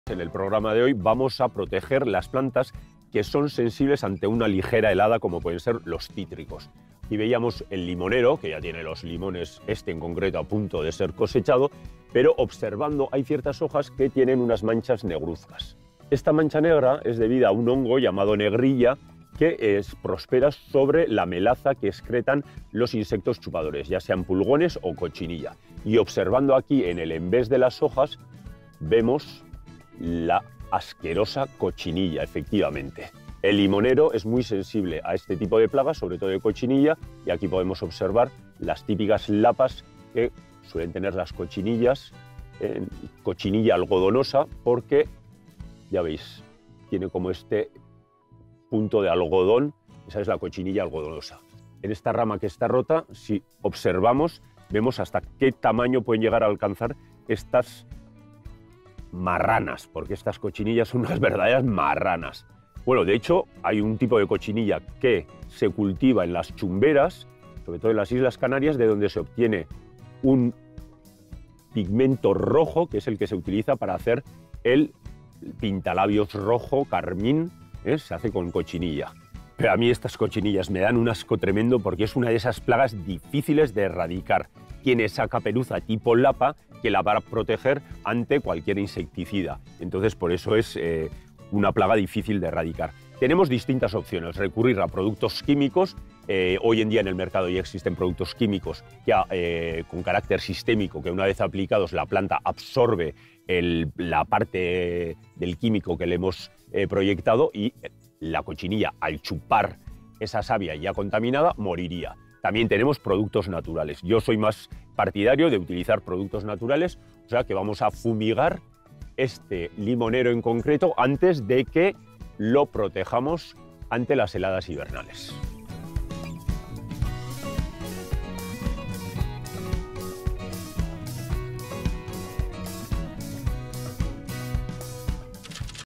Limonero con plaga de cochinilla algodonosa - Decogarden
En nuestro jardín tenemos un limonero plantado en una hermosa maceta. Como es época de heladas y fríos, es conveniente proteger el cítrico.

Además, hemos encontrado algunas hojas del limonero de color negro. Si observamos que las hojas tienen manchas negras, lo más probable es que sea por un hongo llamado melaza o negrilla. Generalmente suele ser producido por insectos chupadores como cochinilla algodonosa o por los pulgones.

Os contamos una forma natural de acabar con ellas. ¡No te lo pierdas!

🌼 ¡Comparte tu pasión por la jardinería! 🌼